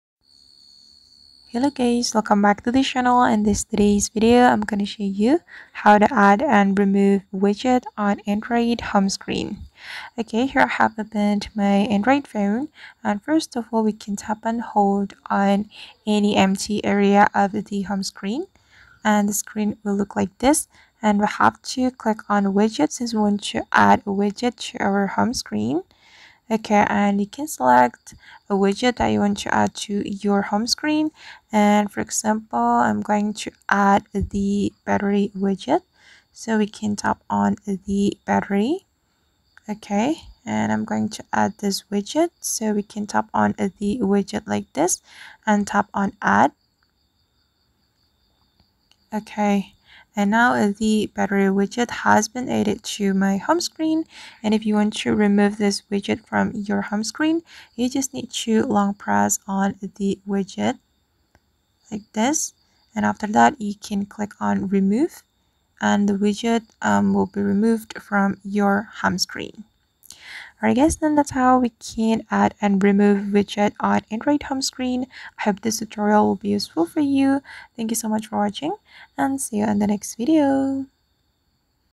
⭐ EASY STEPS: Personalize Your Android Widget! How to Add and Remove Widget on Your Home Screen?
Unlock the full potential of your Android device with our easy-to-follow tutorial on personalizing your widgets! Widgets are a fantastic way to customize your home screen, providing quick access to information and enhancing your overall user experience.